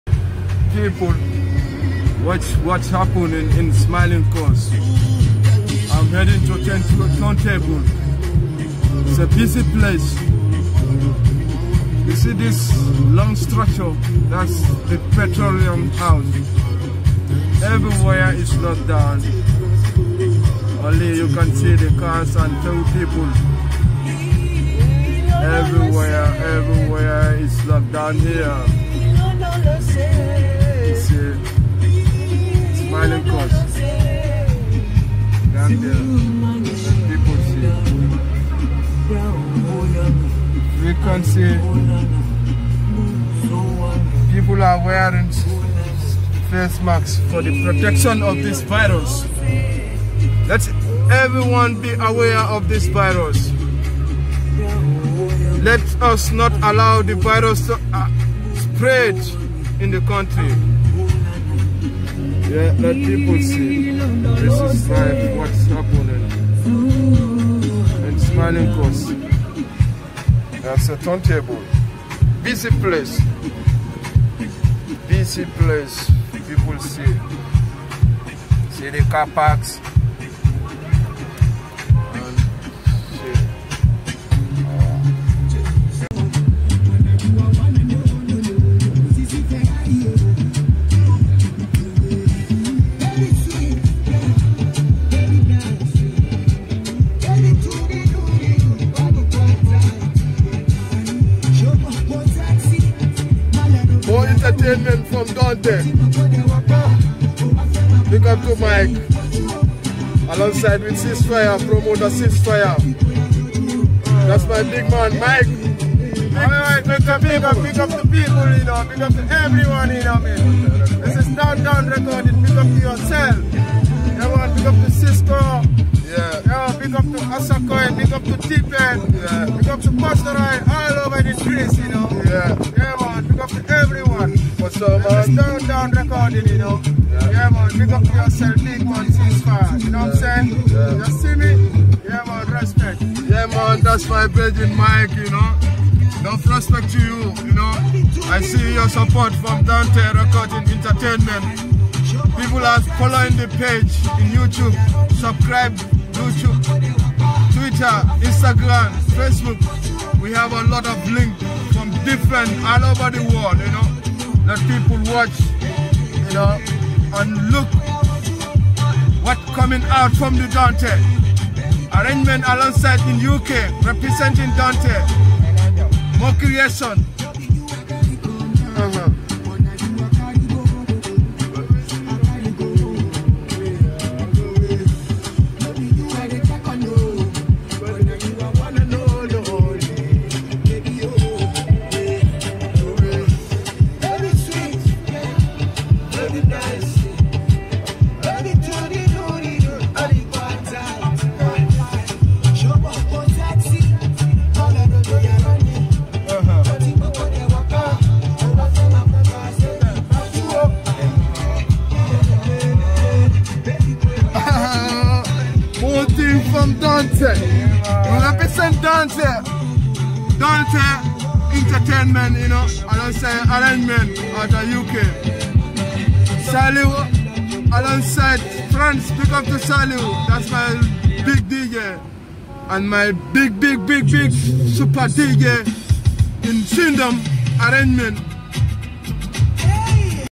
Cease Fyah & Mike (Gambia Lock Down) Senegambia
Dont'e Recording promoter Cease Fyah & Mike (Gambia Lockdown) In Senegambia & Brusubi Rounded Bar - 2020
(Freestyle) live at Dont'e Recording Studio🎙❤💚💛👍🏿🏃🏿‍♂️